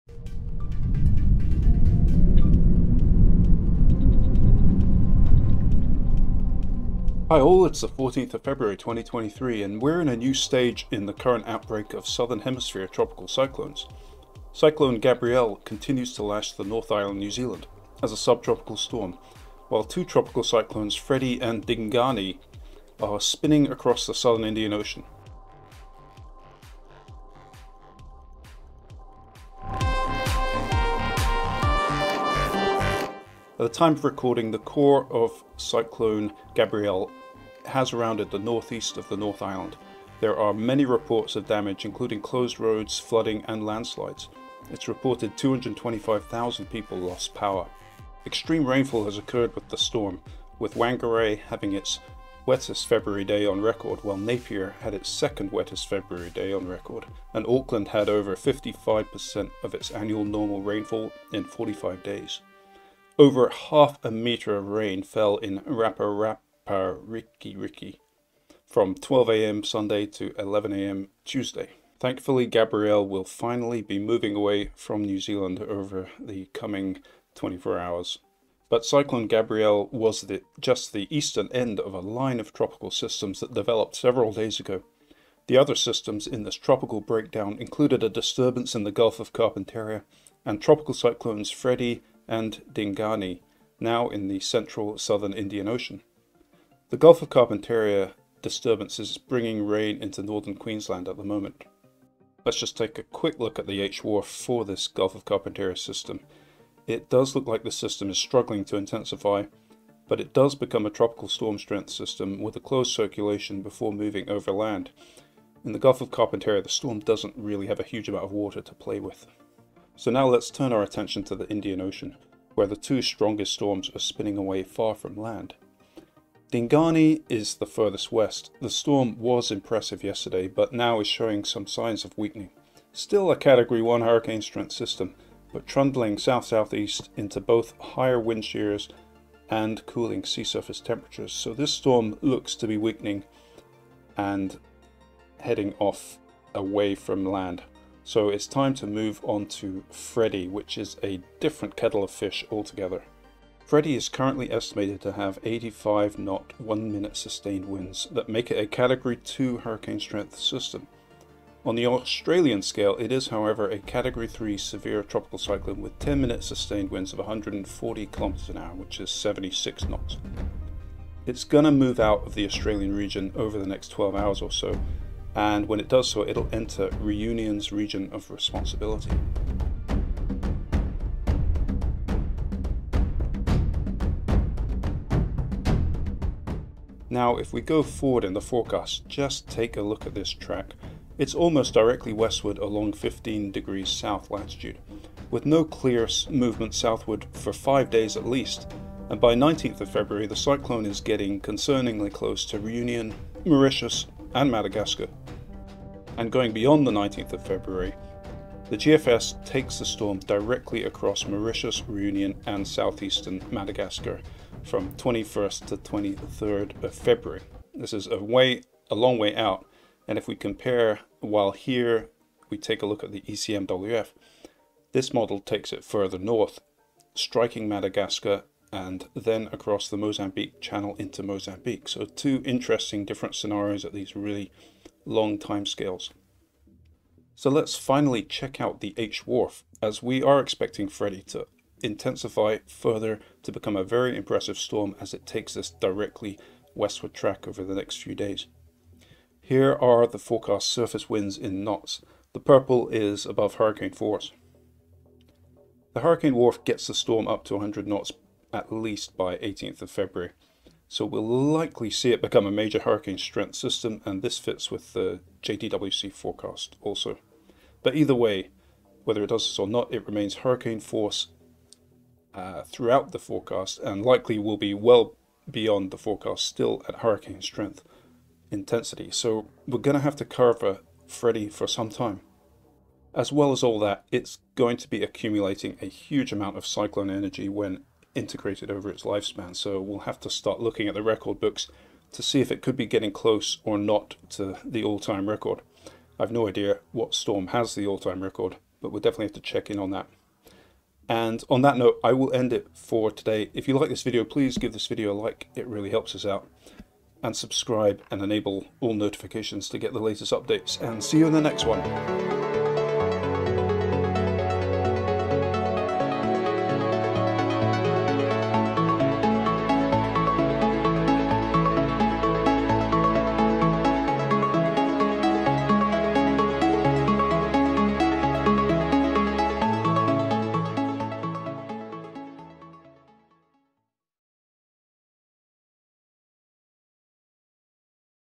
Cyclone Gabrielle and long-track Freddy
Cyclone outlook looks at Gabrielle still lashing North Island New Zealand as well as Tropical Cyclone Dingani but more interestingly Freddy which looks like an extremely long-track tropical cyclone eventually potentially reaching close to Reunion and Madagascar